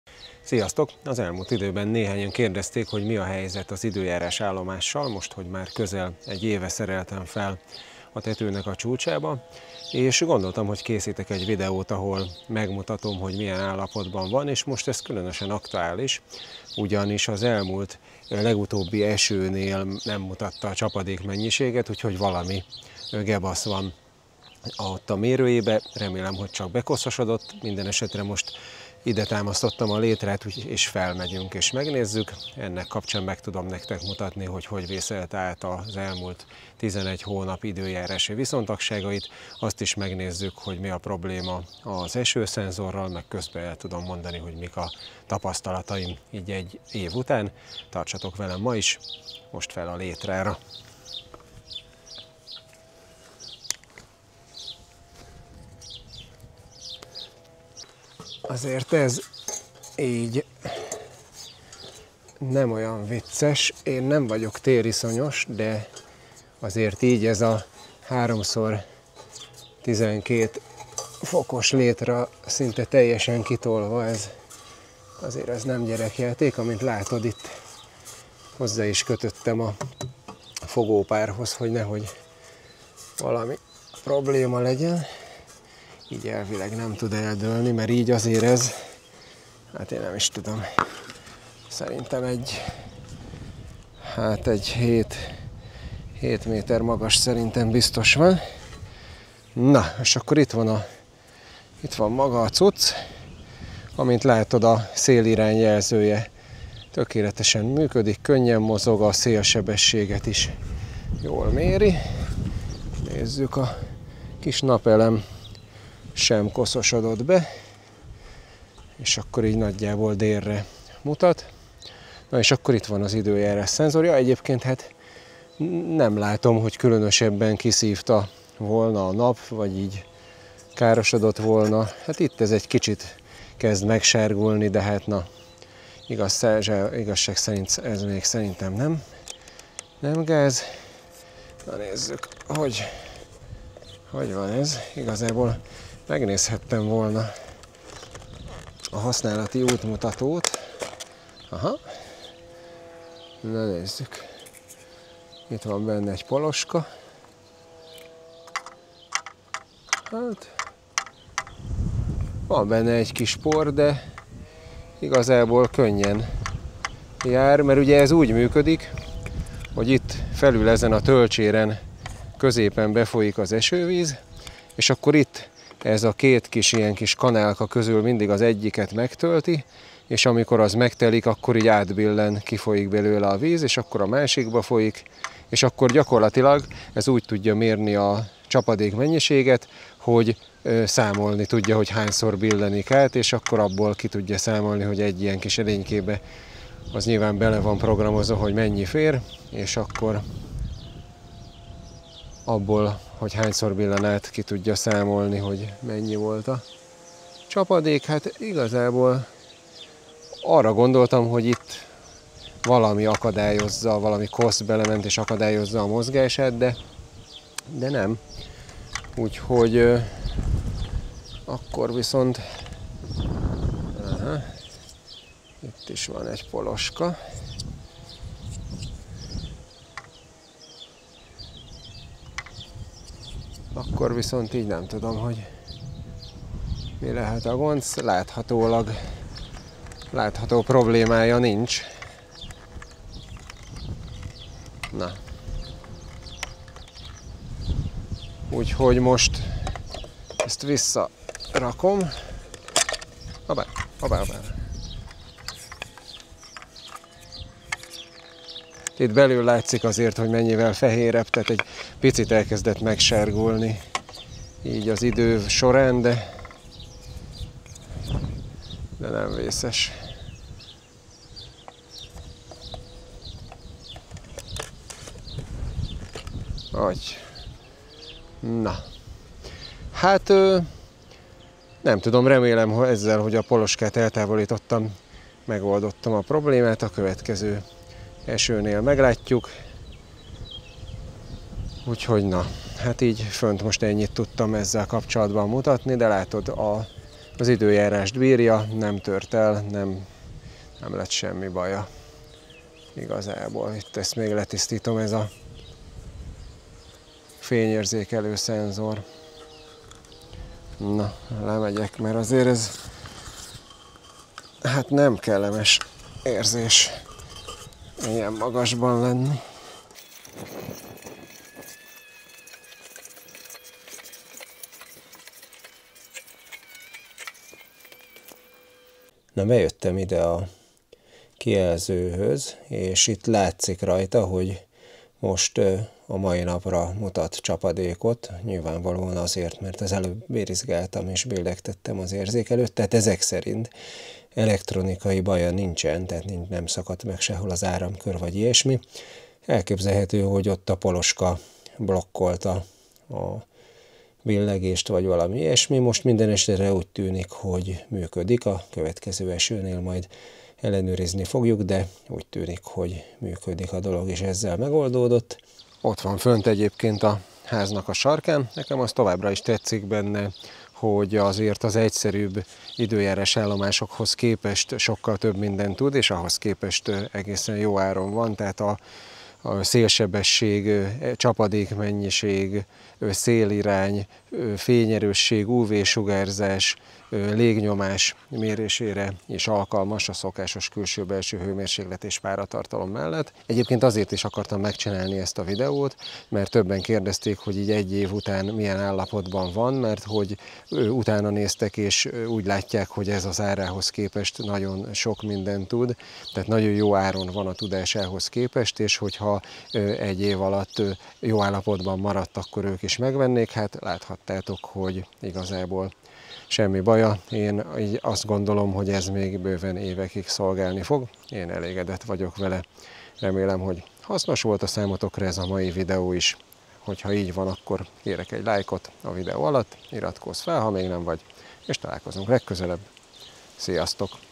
Vevor 7 az 1-ben időjárás állomás tapasztalatok vélemény egy év használat után
Külső - belső hőmérséklet és páratartalom, légnyomás, fény és UV erősség, szélsebesség, szélirány, csapadékmennyiség. Mindezt méri és mindenféle bontásban egy színes beltéri egységen meg is jeleníti az az időjárás állomás, amt már közel egy éve használok. Most megmutatom, milyen állapotban van ennyi idő után, és elhárítom a csapadékmennyiség mérő hibáját is.

A videó alatt a "Köszönet" gombra kattintva közvetlenül is támogathatsz, hogy még jobb videókat készíthessek. Köszi előre is!